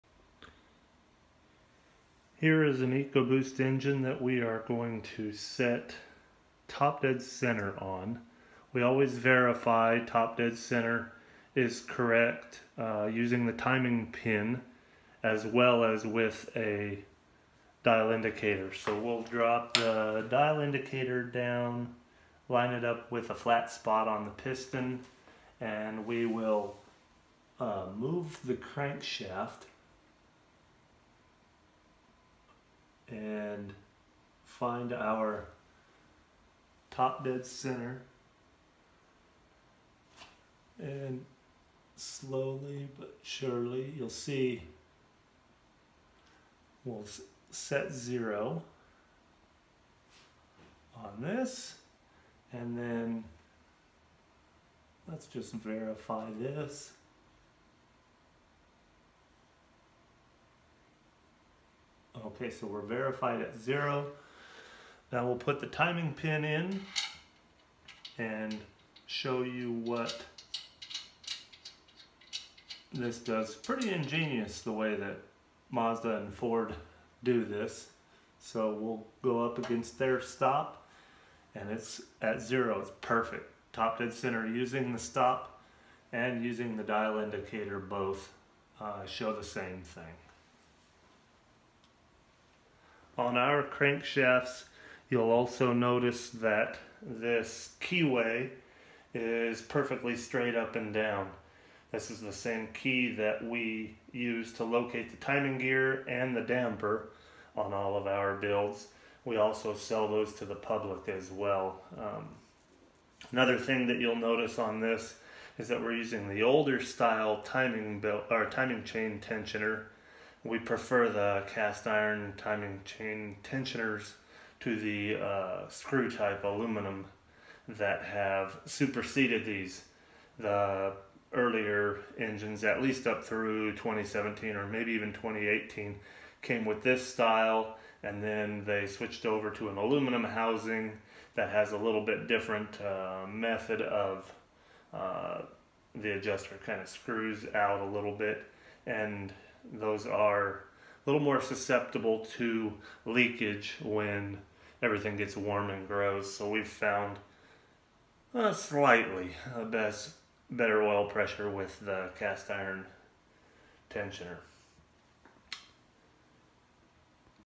How to set TDC on an Ecoboost shortblock
In this video we show you how to set and verify TDC (top dead center) on an Ecoboost shortblock.  These methods are the exact same for Ford Duratec and Mazda MZR engines as well.